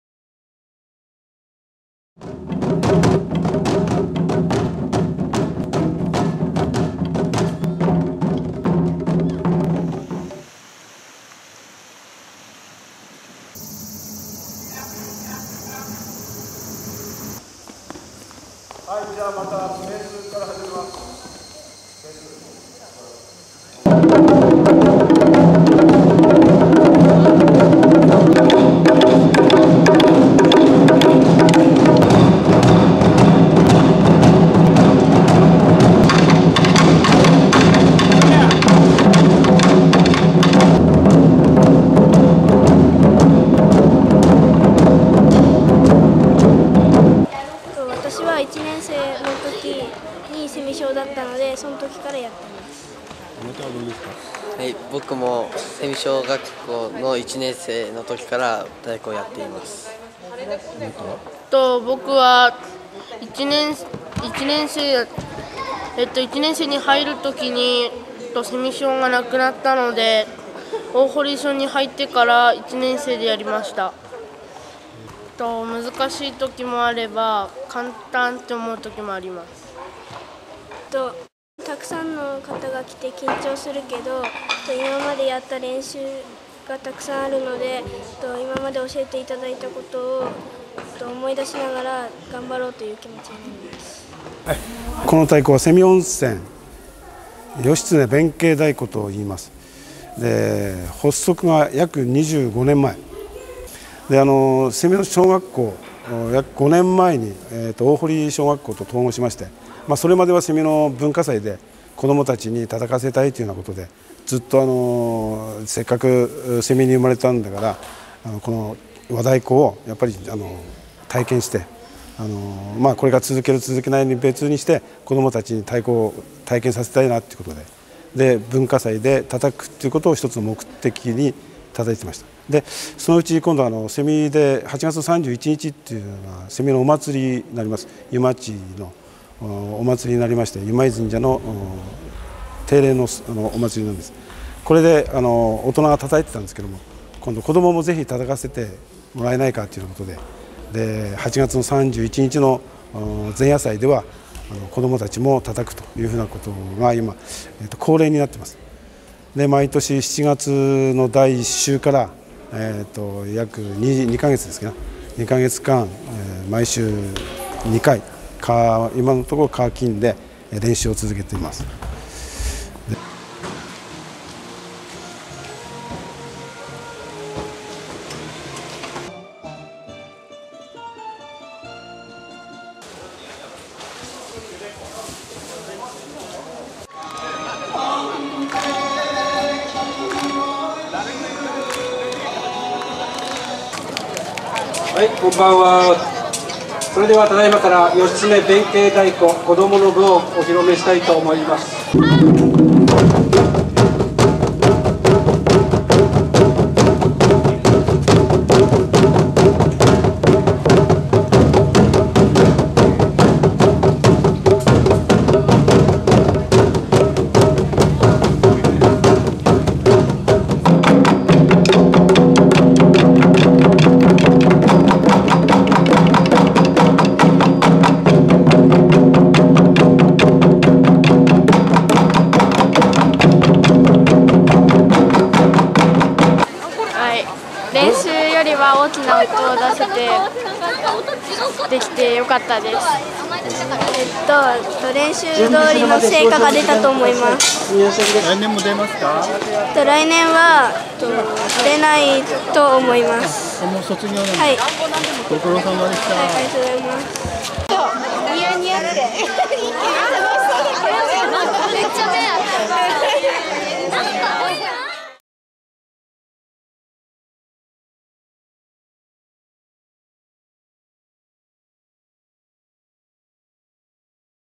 瀬見伝統芸能保存会太鼓部会
平成２年に創設した「義経弁慶太鼓」は、瀬見温泉にゆかりのある義経と弁慶の歴史ロマンを、軽やかでかつ力強い太鼓の響きで表現する創作和太鼓です。
　小学生への継承活動を積極的におこなっており、毎年恒例の瀬見祭り前夜祭で披露するため、お祭り前は２ヶ月間の練習会（計15回）を行っています。
　子どもたちの練習成果は、瀬見祭りや統合した大堀小学校文化祭で披露しており、地域の一員としての自信と誇り、地域への愛着心を育んでいます。